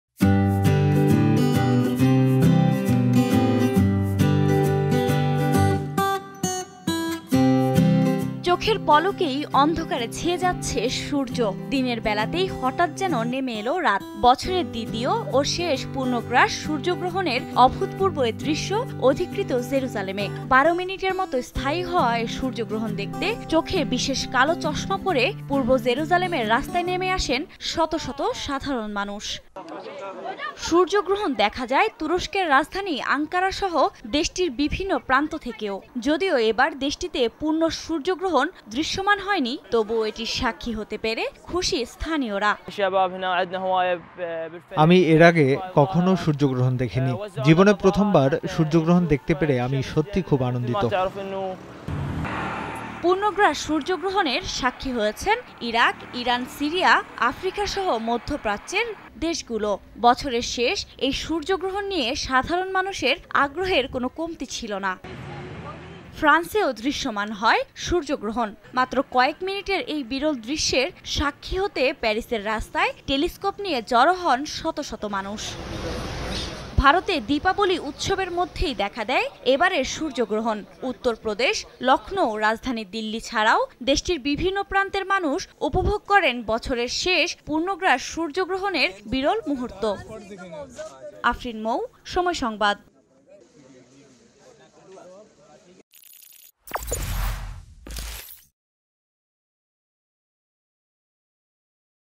দিনের বেলাতেই হঠাৎ যেন নেমে এলো রাত | Solar Eclipse | Total solar eclipse 2022 | Somoy TV
বিশ্বের বিভিন্ন দেশে দেখা গেছে পূর্ণগ্রাস সূর্যগ্রহণ। সূর্যগ্রহণের সাক্ষি হতে মঙ্গলবার রঙিন চশমা ও টেলিস্কোপ নিয়ে বিশ্বের বিভিন্ন দেশের রাস্তায় ভিড় করেন সাধারণ মানুষ। সূর্যগ্রহণটি ইউরোপ ও এশিয়ার বিভিন্ন প্রান্ত ছাড়াও, উত্তর আফ্রিকার কয়েকটি দেশ থেকে দেখা যায়।